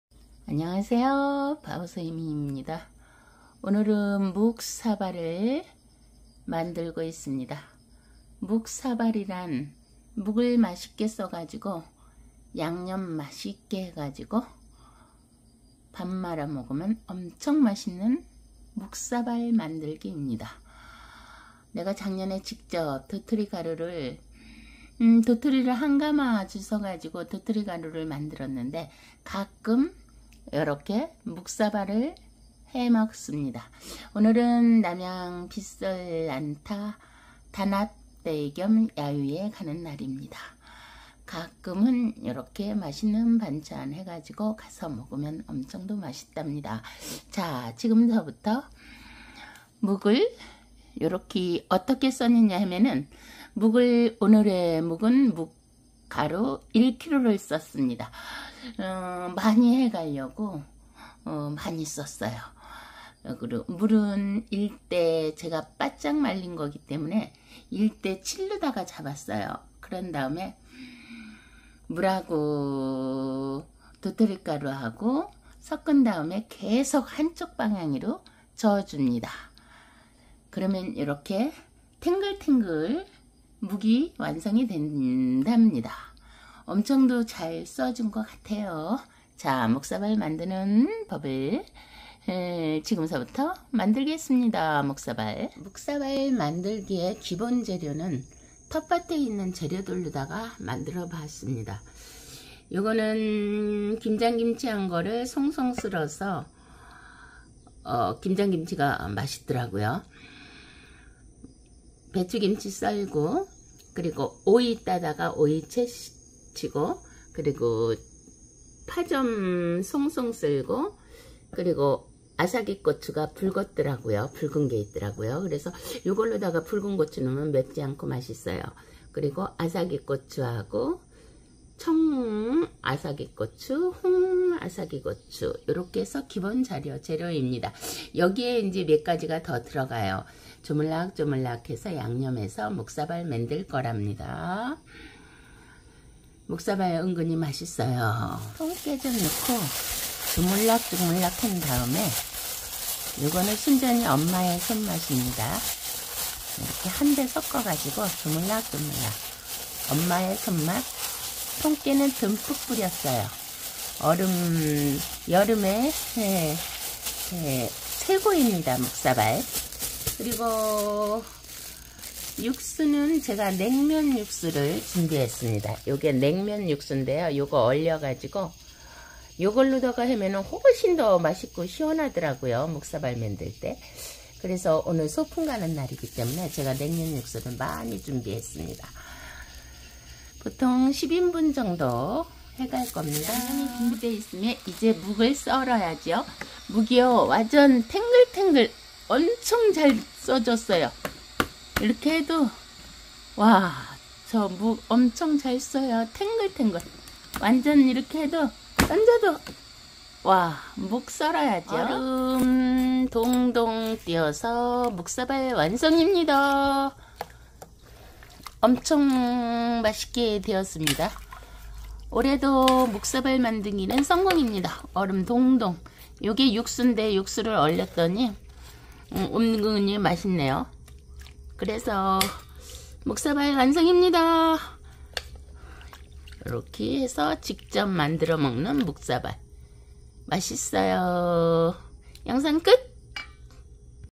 묵사발 단합대회 남양빛솔난타 소풍가는날 야유회 도토리묵
답합대회
오이지,
상치,
아삭이고추,
묵사발,
도토리묵,
깻잎김치,
고구마줄기,
쌈장,
금화규꽃밥 준비 완료
~삼겹살구이
남양행정복지센터
야간난타반
동아리
남양빛솔난타
천안 성거산  천흥계곡
단합대회 워크샵
야우회 소풍가는날
내가 직접 도토리줏어서
가공한 도토리가루
1kg묵 맛있게 쑤어서
묵사발 만들면서
핸드폰으로
영상 셀카 찍었다
늘
남양빛솔난타
기부봉사도 다니며
함께 봉사하는 난타반
화이팅이다 !!!